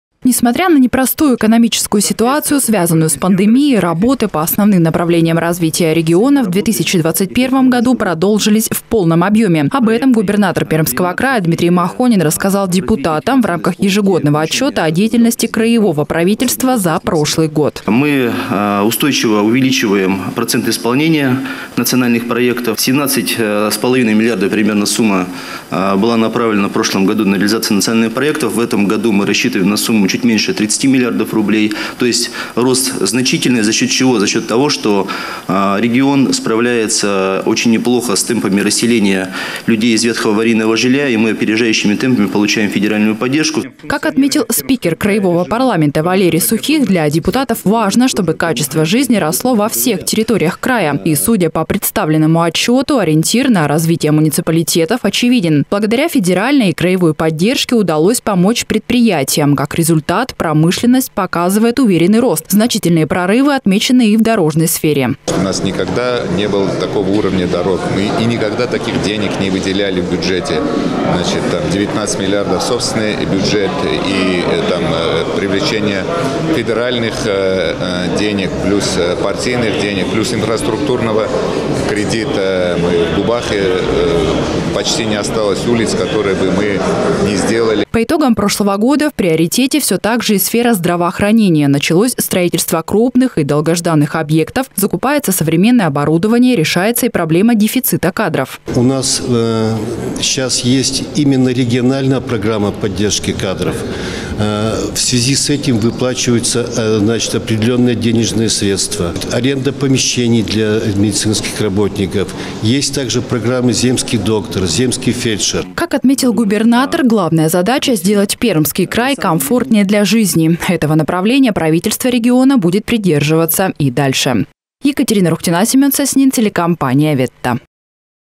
Итоги работы правительства
Национальные проекты, здравоохранения, дороги и уровень жизнь – губернатор Пермского края отчитался перед депутатами за работу правительства в прошлом году. Какие проекты реализуются в крае, расскажут наши коллеги из телекомпании "Ветта".